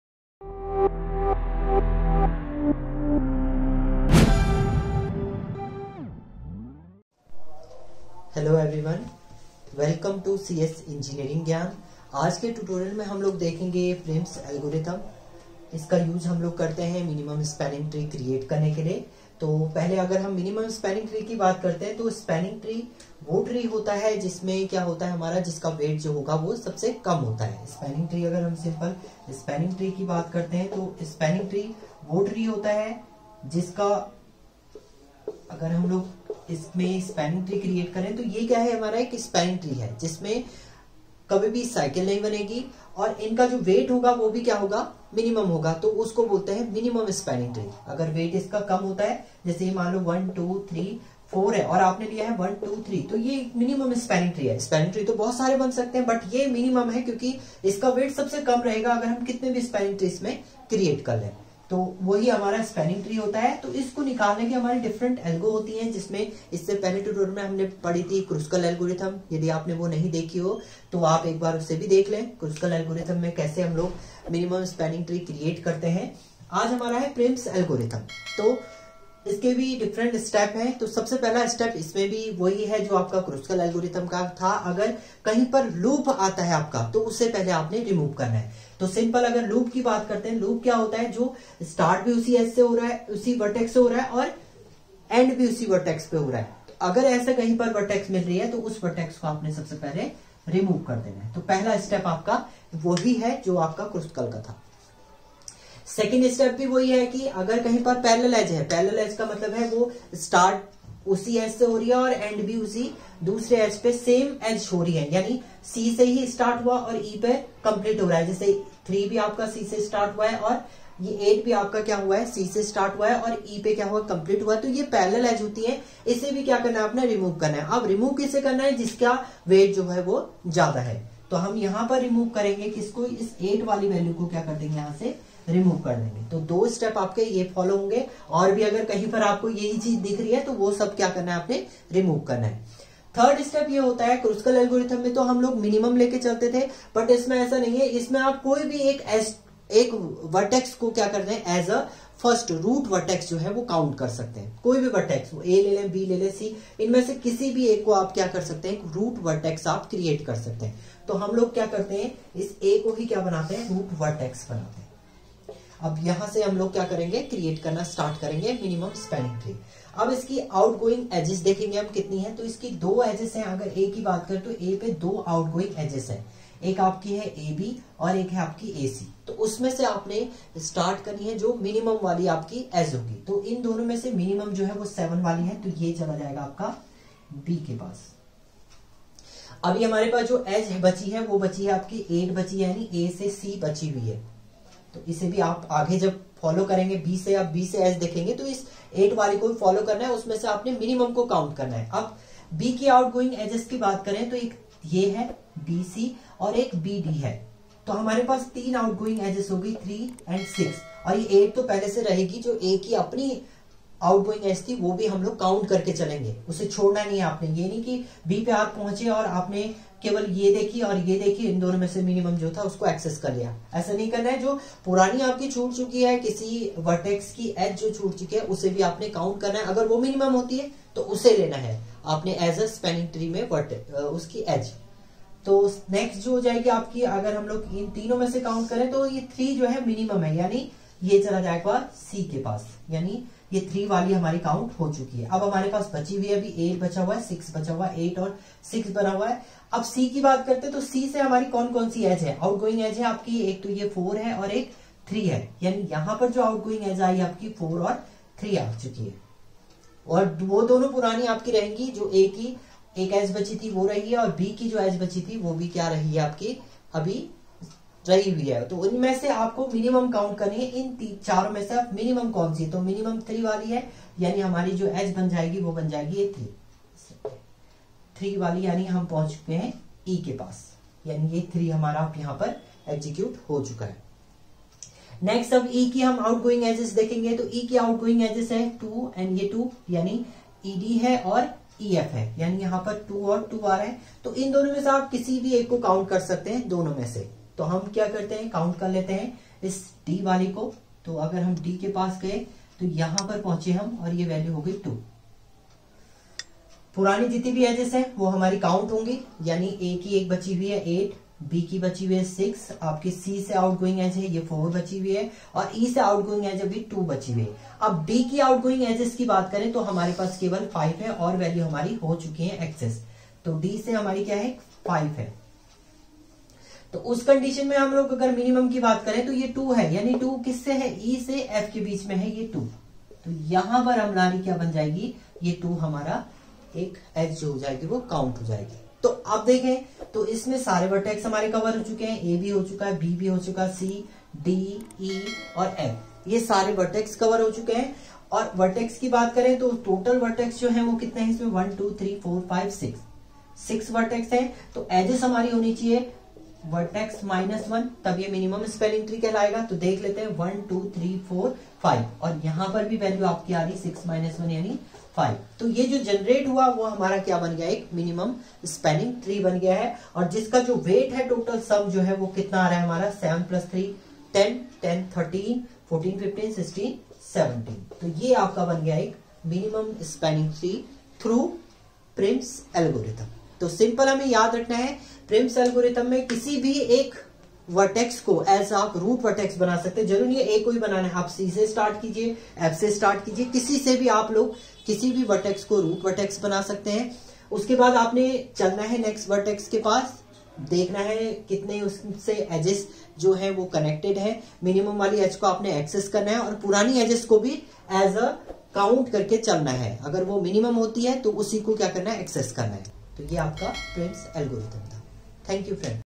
Prim's algorithm for minimum spanning trees in Hindi ( Example ) | Data Structure
Prim's algorithm in data structure with example in Data Structure | Minimum tree in Hindi
Prims & Kruskals Algorithms - Greedy Method

BTech syllabus
polytechnic course IT

CSE Gyan
Cs Engineering Gyan
Teacher: Kailash Joshi
Editor: Bipin Chandra